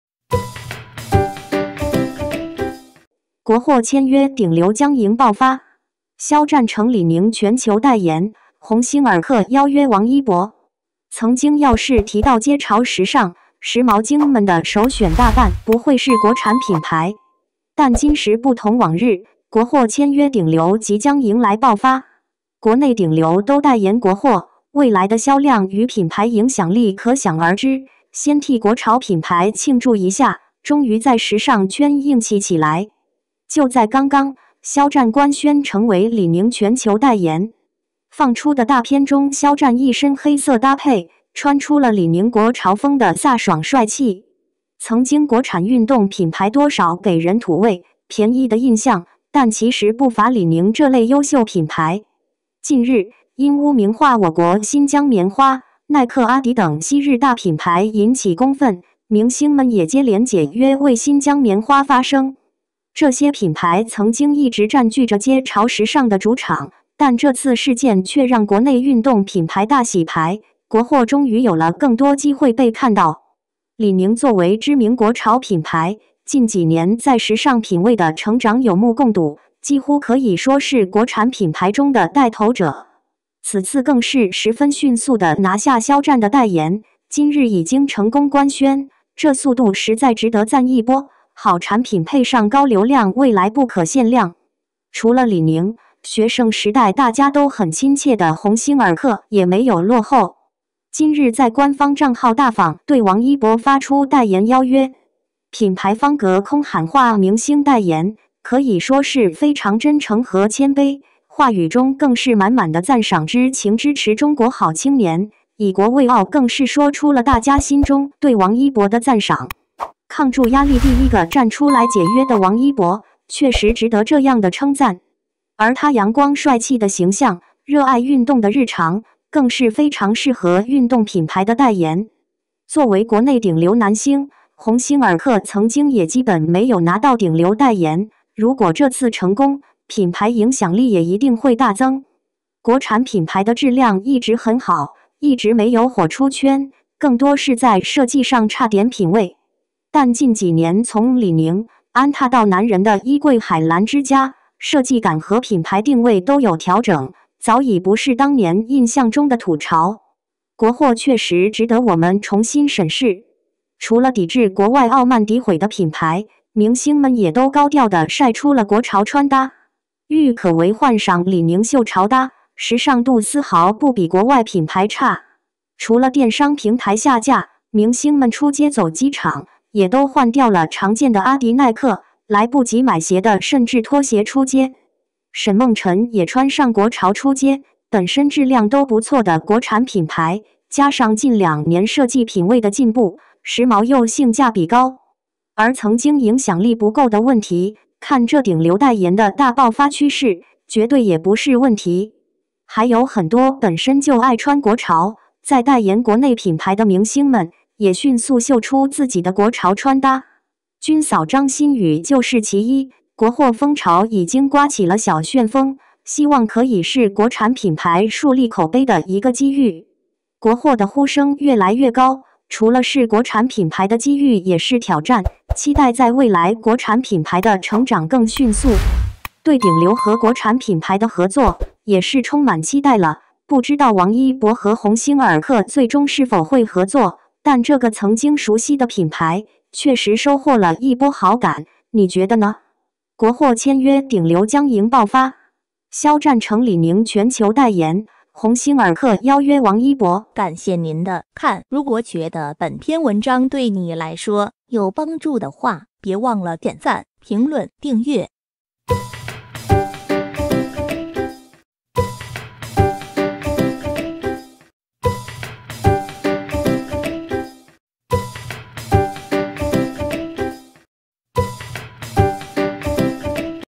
ENG SUB - 国货签约顶流将迎爆发！肖战成李宁 全球代言，鸿星尔克邀约#王一博【陈氏日报】
国货签约顶流将迎爆发！肖战成李宁 全球代言，鸿星尔克邀约#王一博。
曾经要是提到“街潮时尚”，时髦精们的首选大半不会是国产品牌。
但今时不同往日，国货签约顶流即将迎来爆发！
国内顶流都代言国货，未来的销量与品牌影响力可想而知，先替国潮品牌庆祝一下，终于在时尚圈硬气起来！
就在刚刚，肖战官宣成为李宁全球代言。放出的大片中肖战一身黑色搭配，穿出了李宁国潮风的飒爽帅气。曾经国产运动品牌多少给人土味、便宜的印象，但其实不乏李宁这类优秀品牌。
近日，因污名化我国新疆棉花，耐克阿迪等昔日“大品牌”引起公愤，明星们也接连解约为新疆棉花发声。
这些品牌曾经一直占据着街潮时尚的主场，但这次事件却让国内运动品牌大洗牌，国货终于有了更多机会被看到。
李宁作为知名国潮品牌，近几年在时尚品味的成长有目共睹，几乎可以说是国产品牌中的带头者。
此次更是十分迅速的拿下肖战的代言，今日已经成功官宣，这速度实在值得赞一波，好产品配上高流量未来不可限量。
除了李宁，学生时代大家都很亲切的“鸿星尔克”也没有落后。今日在官方账号大方对王一博发出代言邀约。
品牌方隔空喊话明星代言，可以说是非常真诚和谦卑，话语中更是满满的赞赏之情“支持中国好青年，以国为傲”更是说出了大家心中对王一博的赞赏。
抗住压力第一个站出来解约的王一博，确实值得这样的称赞。而他阳光帅气的形象，热爱运动的日常，更是非常适合运动品牌的代言。
作为国内顶流男星，鸿星尔克曾经也基本没有拿到顶流代言，如果这次成功，品牌影响力也一定会大增。
国产品牌的质量一直很好，一直没有火出圈，更多是在设计上差点品味。但近几年从李宁、安踏到男人的衣柜海澜之家，设计感和品牌定位都有调整，早已不是当年印象中的“土潮”，
国货确实值得我们重新审视。
除了抵制国外傲慢诋毁的品牌，明星们也都高调的晒出了国潮穿搭。郁可唯换上李宁秀潮搭，时尚度丝毫不比国外品牌差。
除了电商平台下架，明星们出街走机场，也都换掉了常见的阿迪耐克，来不及买鞋的甚至拖鞋出街。
沈梦辰也穿上国潮出街，本身质量都不错的国产品牌，加上近两年设计品味的进步，时髦又性价比高。而曾经影响力不够的问题，看这顶流代言的大爆发趋势，绝对也不是问题。
还有很多本身就爱穿国潮、在代言国内品牌的明星们，也迅速秀出自己的国潮穿搭。
军嫂张馨予就是其一，国货风潮已经刮起了小旋风，希望可以是国产品牌树立口碑的一个机遇。
国货的呼声越来越高，除了是国产品牌的机遇也是挑战，期待在未来国产品牌的成长更迅速。对顶流和国产品牌的合作，也是充满期待了，不知道王一博和鸿星尔克最终是否会合作，但这个曾经熟悉的品牌，确实收获了一波好感，你觉得呢？
国货签约顶流将迎爆发！肖战成李宁全球代言，鸿星尔克邀约王一博。

# 名人 粉丝 # 有 名人